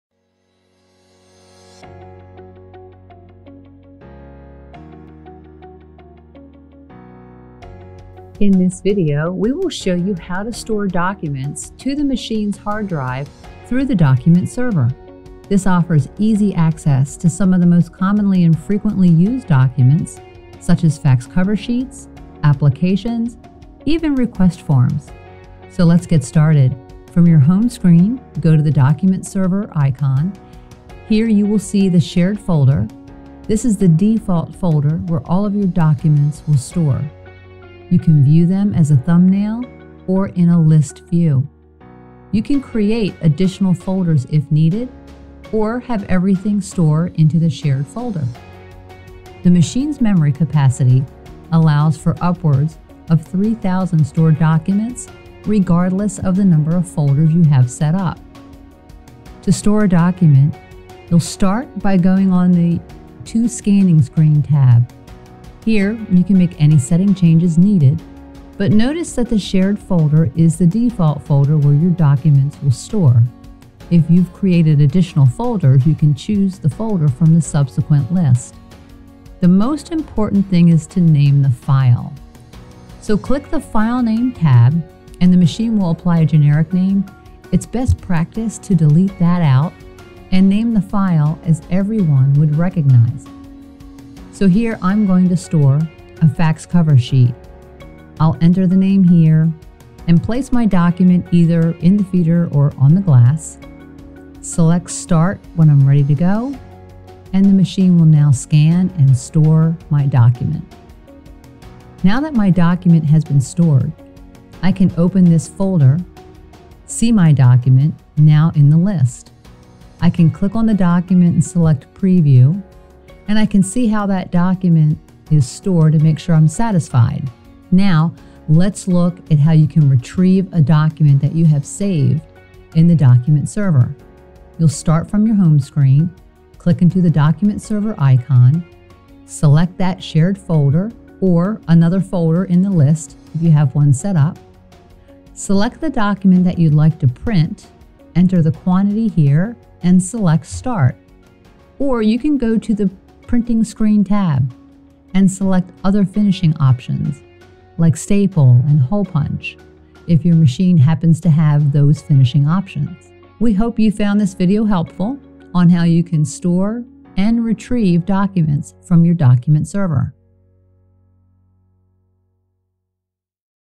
Document Server Features on Your Ricoh Savin MP & IM Series Device
This video explains how to save documents on the copier that are commonly used or recalled on a frequent basis. We will walk you through step-by-step instructions detailing  how to store a document on the copier in the Document Server.

for additional training resources or to ask a question!

Please note: These instructions are pertinent to the following equipment: Savin/Ricoh